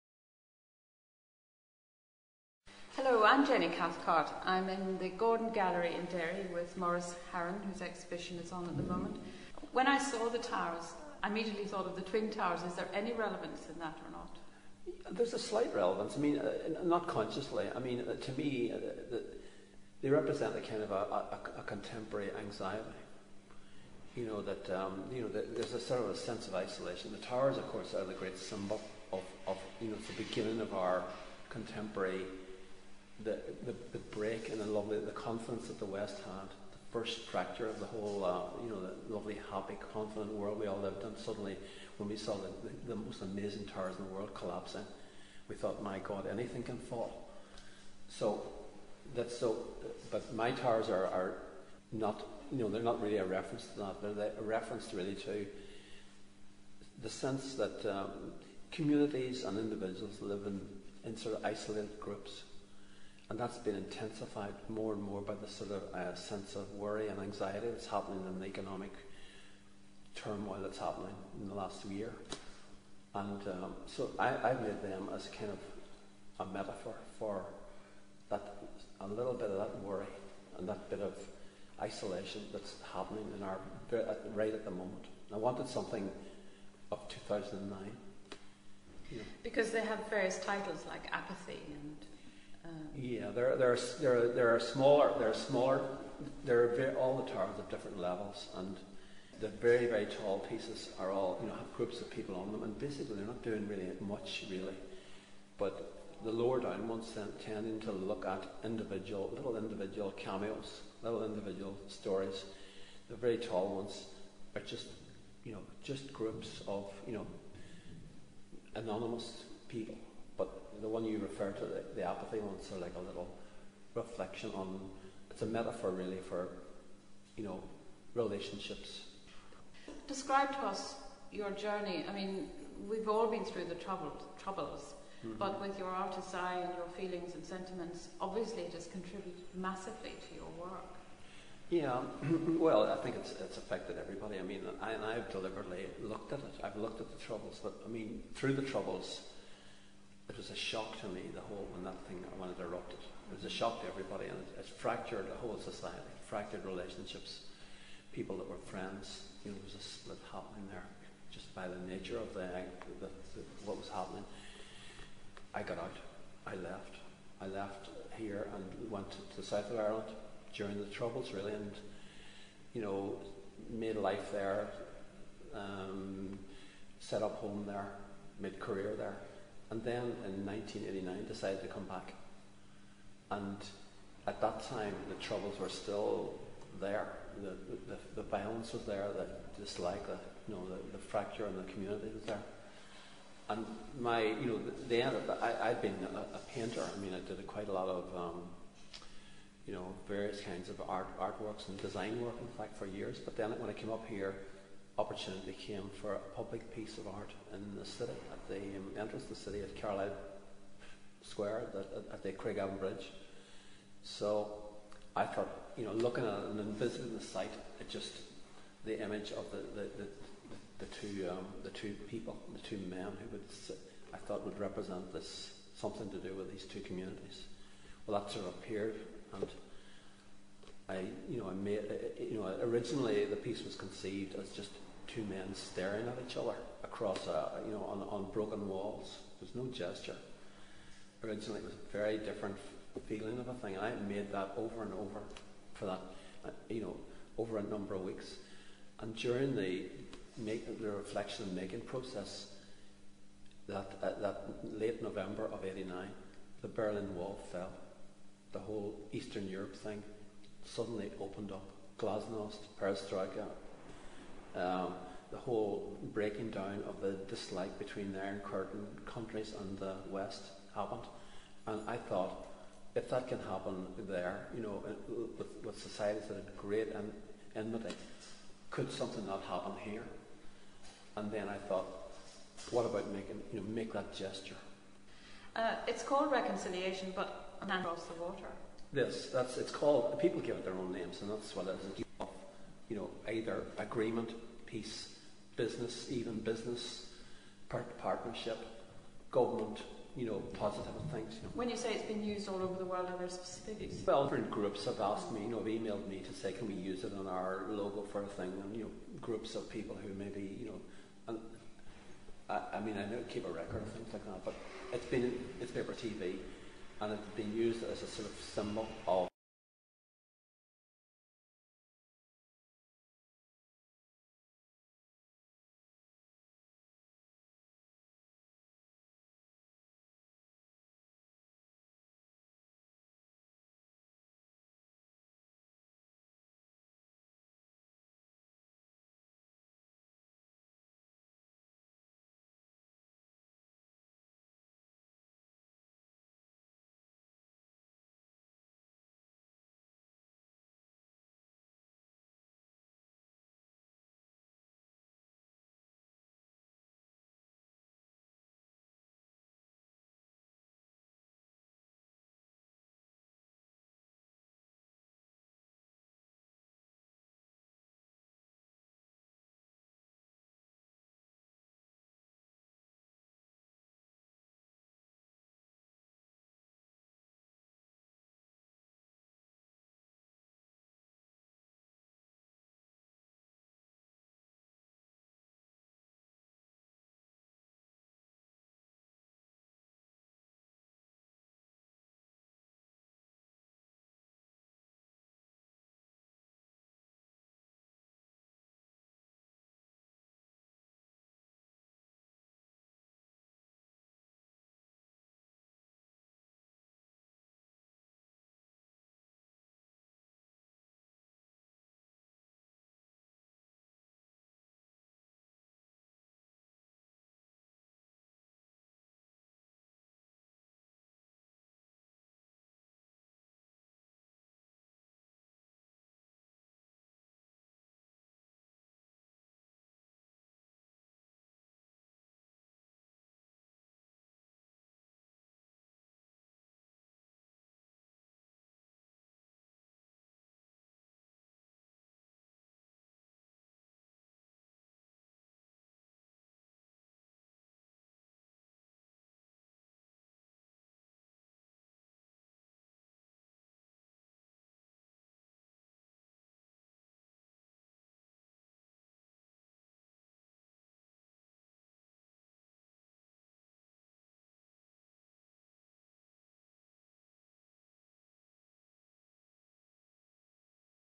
Maurice Harron
CultureNorthernIreland's Jenny Cathcart met up with renounded sculptor Maurice Harron in the Gordon Gallery, Derry, to talk about the artist's first exhibition and his famous work, 'Reconciliation', a symbol of hope in Derry City.